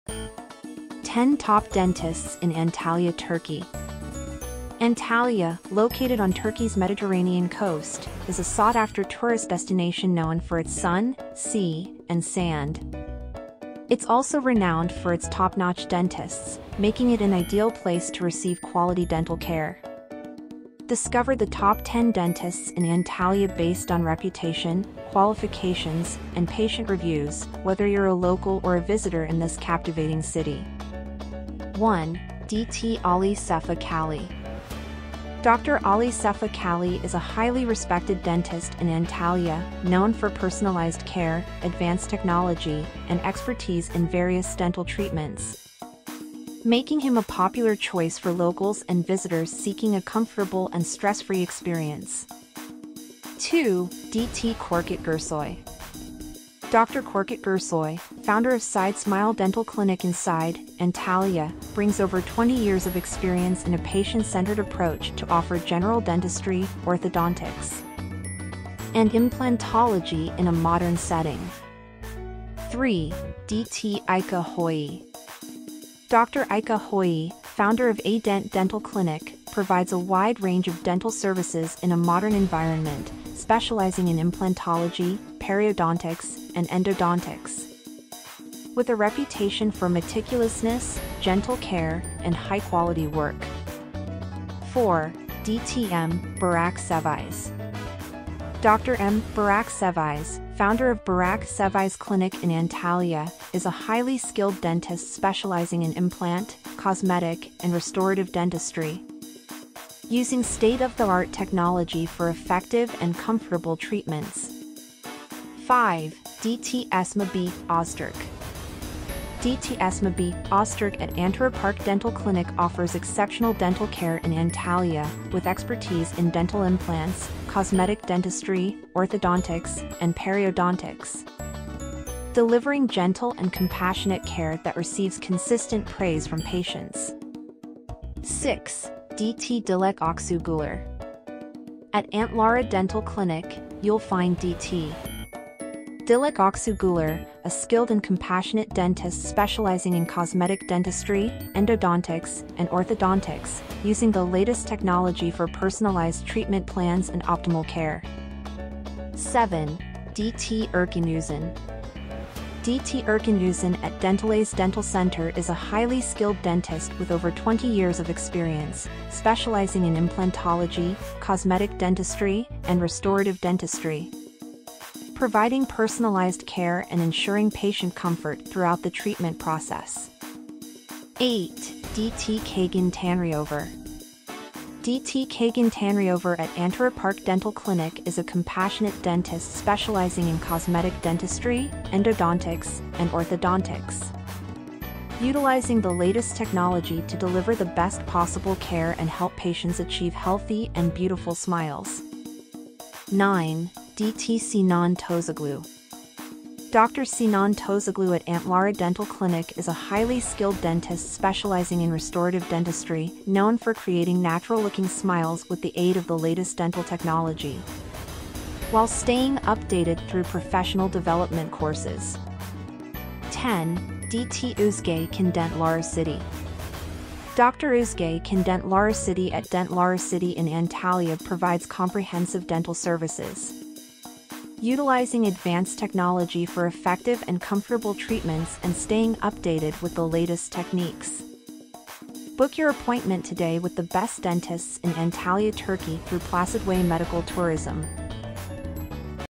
10 Experienced Dentists in Antalya Turkey
Discover the top 10 dentists in Antalya, Turkey, known for their exceptional dental care. From cosmetic dentistry to implantology, find your perfect smile in this captivating city.

1. Dt. Ali Sefa Cali
2. Dt. Korkut Gursoy
3. Dt. Ayca Hoyi
4. Dt. M. Burak Ceviz
5. Dt. Esma B. Ozturk
6. Dt. Dilek Aksu Guler
7. Dt. Erkan Uzun
8. Dt. Kagan Tanriover
9. Dt. Sinan Tozoglu
10. Dt. Ozge Can Dent Laracity

Dental treatment refers to the medical procedures and interventions performed by dentists to diagnose, prevent, and treat various dental conditions and oral health issues. Dental treatments encompass a wide range of procedures, including general dentistry, cosmetic dentistry, orthodontics, implantology, periodontics, endodontics, and more.

Antalya, Turkey, is a popular choice for dental treatment for several reasons. First and foremost, Antalya is renowned for its exceptional dentists who possess high levels of expertise, qualifications, and experience. Many dentists in Antalya have received international education and training, ensuring they stay updated with the latest advancements in dental techniques and technologies.